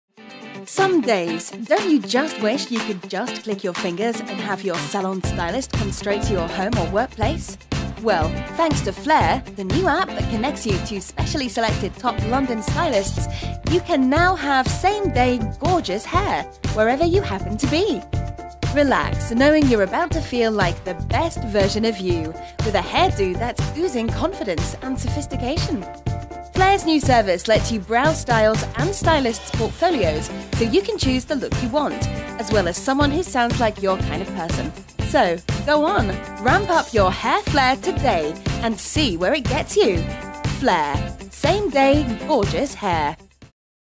Flair, same day gorgeous hair.
An on demand service turbo-charging hairstyling - bringing it up-to-speed with the lives who know great style is mandatory.

Flair is making gorgeous hair and the confidence that brings available to busy young professionals on the day they need it, where they need it. Technology has enabled us to move fast both at work and socially
and set the style bar higher than ever.

Flair wants to bring hairdressing up-to-speed and make high quality stylists as readily available as taxis, dates and good food.

Through an on-demand app, London’s top hair stylists will be able to upload their portfolios and begin a trusted relationship
with a whole new customer base.